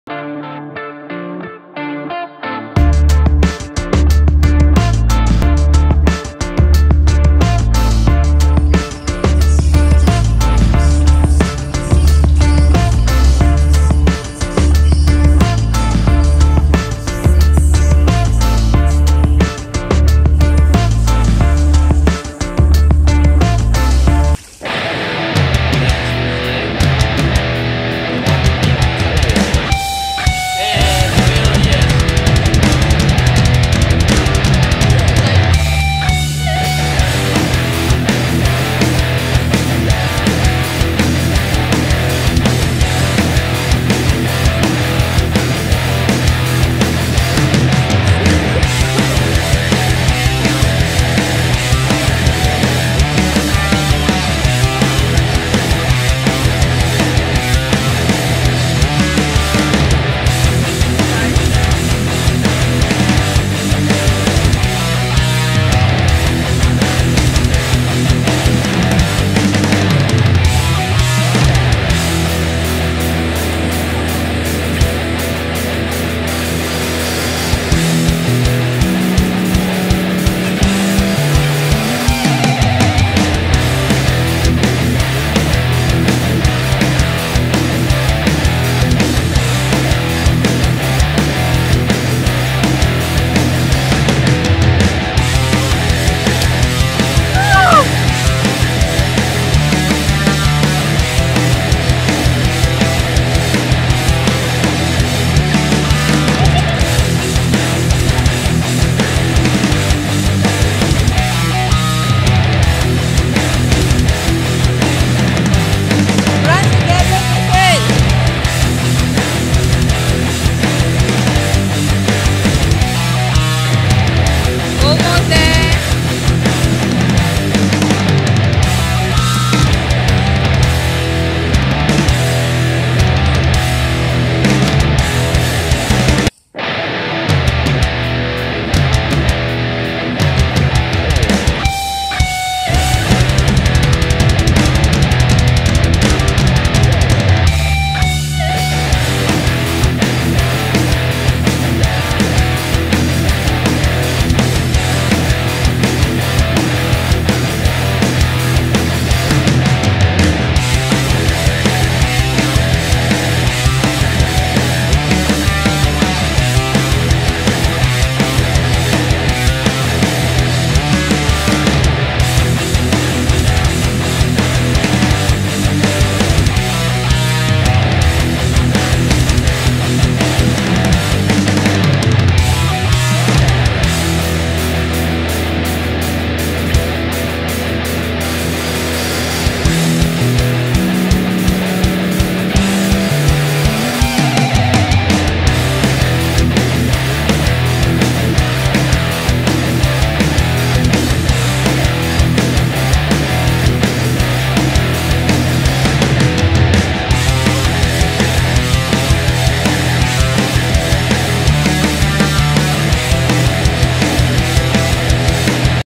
Stealth Beach Summer Camp 4-6 Dec 16 - Wonderful Memories - Highlights
The Stealth Organisation Malaysia Beach Summer Camp 2016, in Corus Paradise Resort Port Dickson

Video credit to: The Stealth Organisation Malaysia